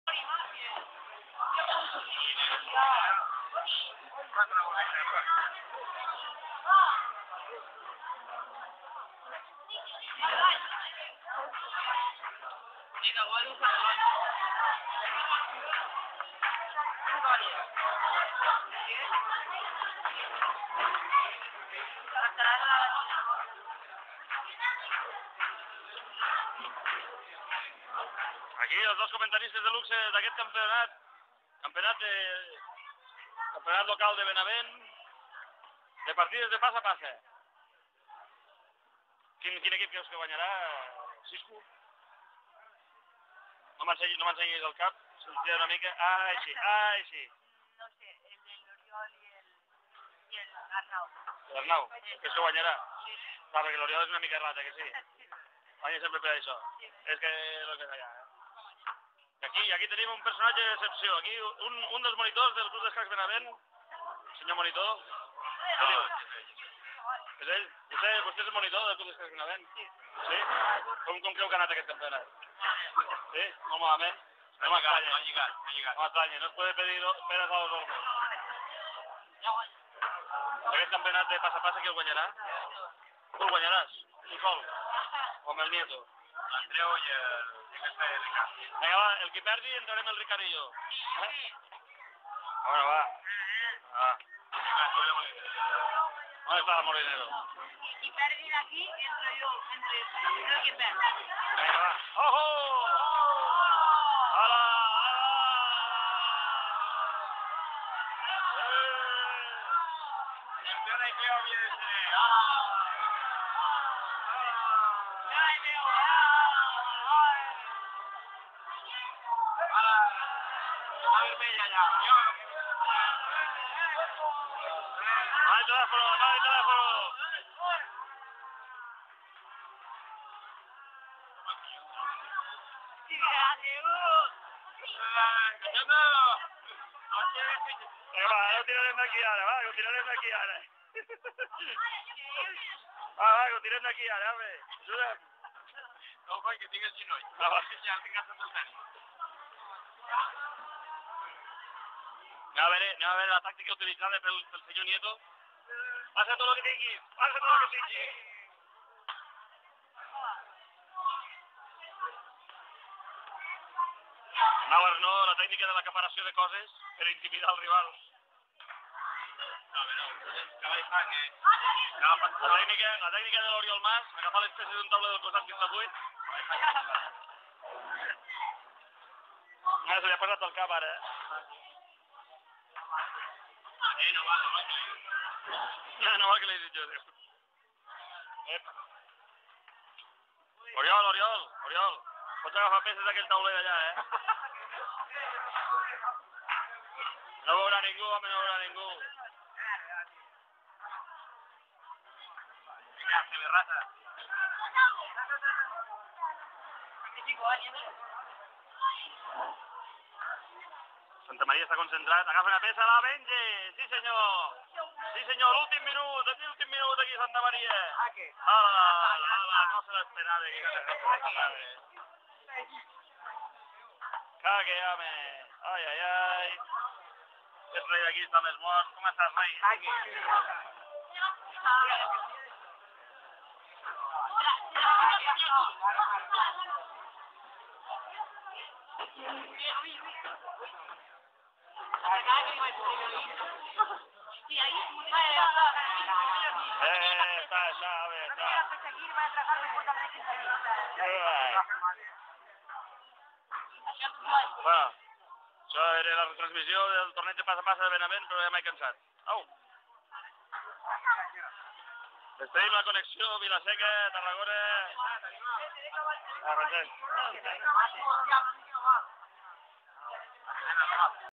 Passa peces home!!! Passa peces!!!
Els xavals de Benavent, que volien lligar i no sabien com fer-ho... pobres noies!!!